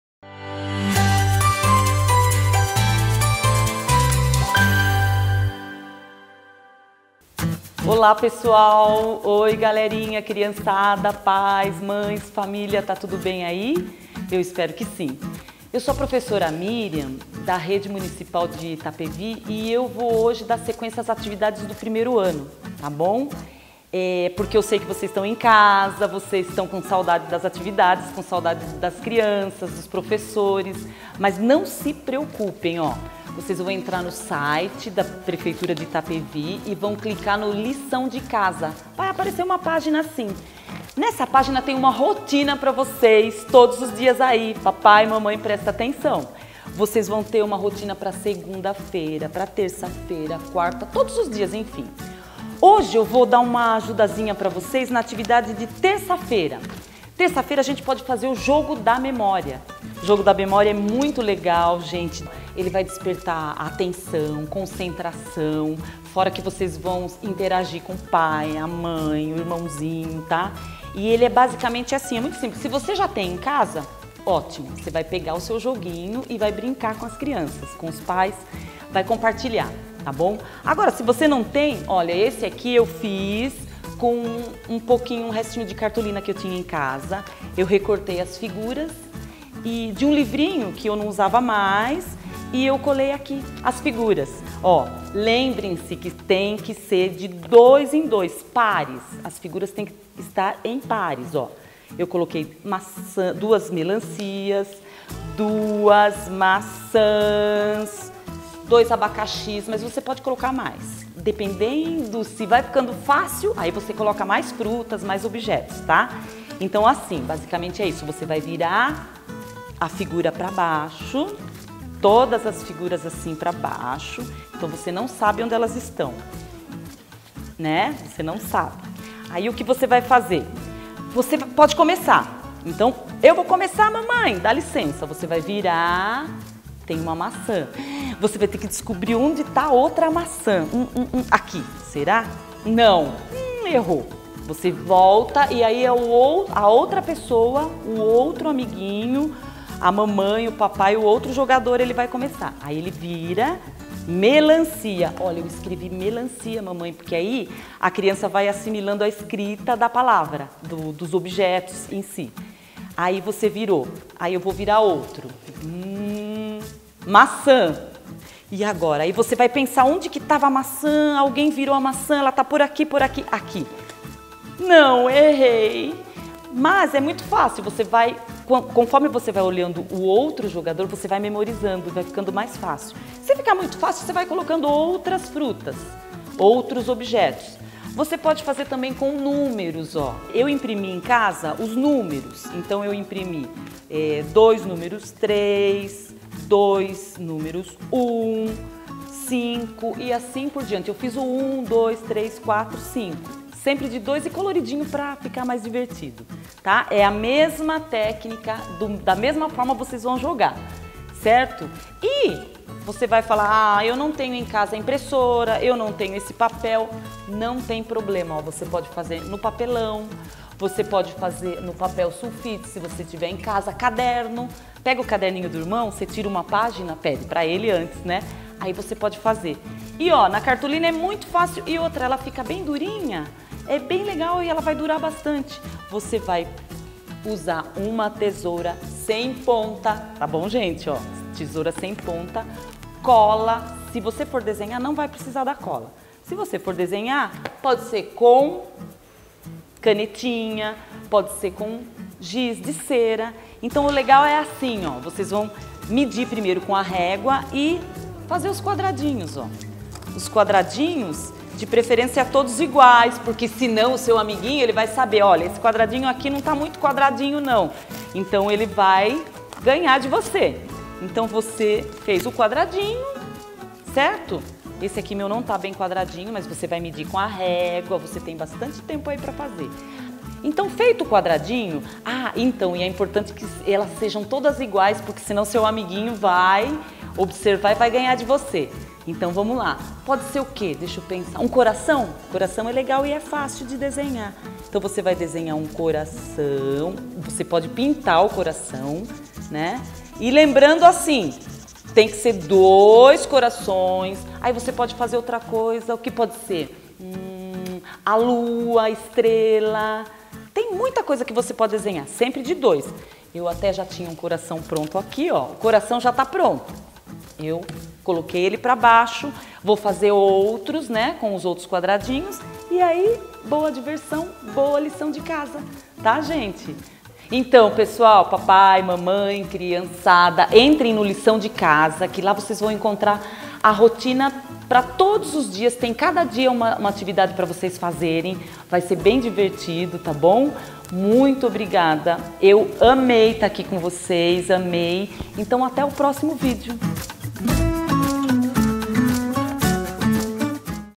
Lição de Casa Aula 01 para alunos do 1º Ano com Professora Mirian
Acompanhe a primeira aula para os alunos dos primeiros anos, com a Professora Mirian.

Aproveite e fique em casa.